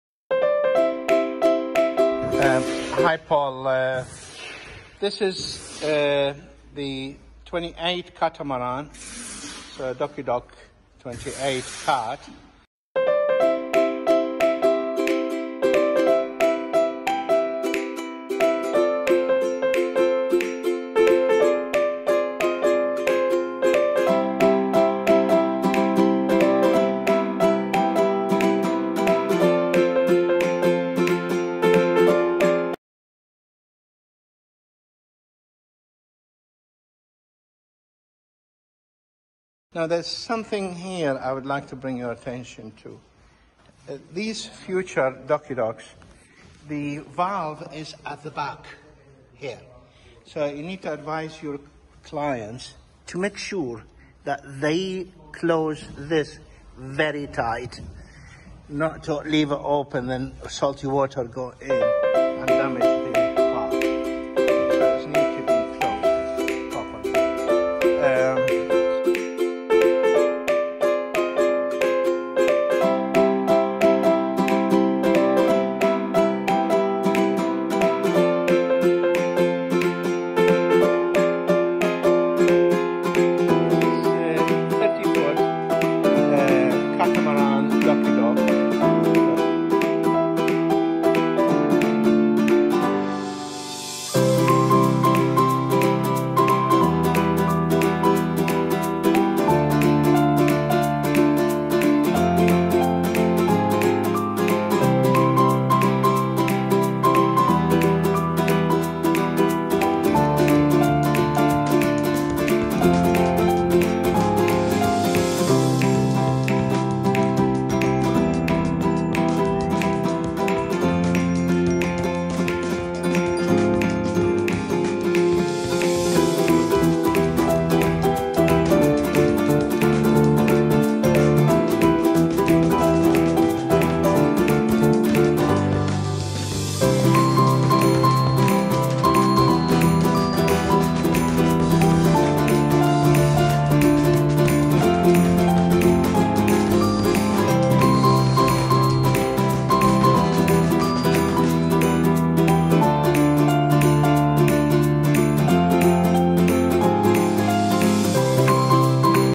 DockyDock CAT development for Catamaran and pontoon boats
The DockyDock Hull cover is a protective cover for the boat hull, protecting it from barnacles, algae and sea creatures. Developed by Direct Linen Textiles International Ltd.
Project Director Yaseen Al-Basri, Manufacturing manager Qasim Al-Basri.